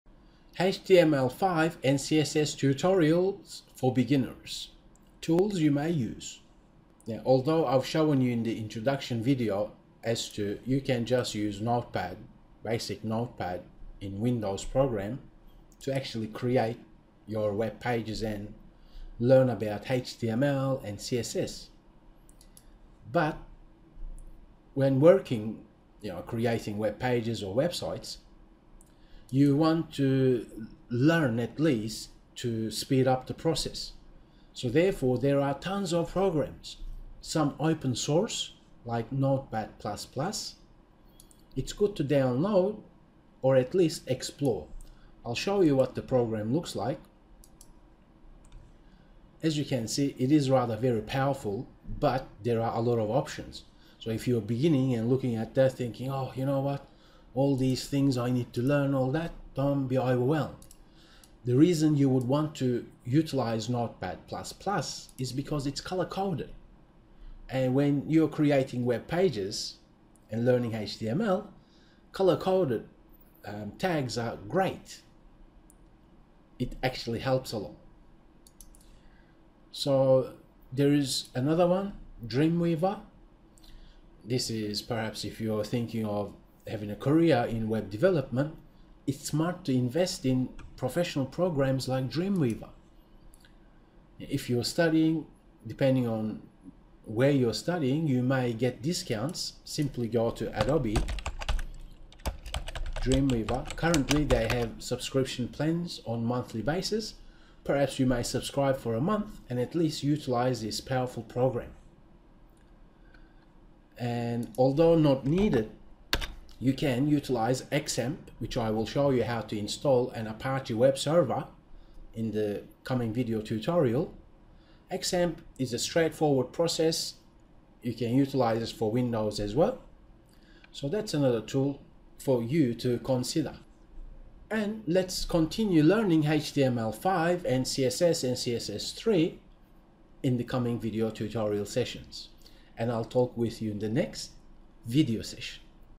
Tools You May Use - html5 & CSS Course - Full html5 Course for FREE
HTML5 and CSS3 Tutorials for Beginners Tools You May Use for web development.

World Wide Web Consortium (W3C) WWW Standards:
W3C develops these technical specifications (including, HTML, HTML5, CSS, CSS3) and guidelines through a process designed to maximize consensus about the content of a technical report, to ensure high technical and editorial quality, and to earn endorsement by W3C and the broader community.

Full html5 Course for free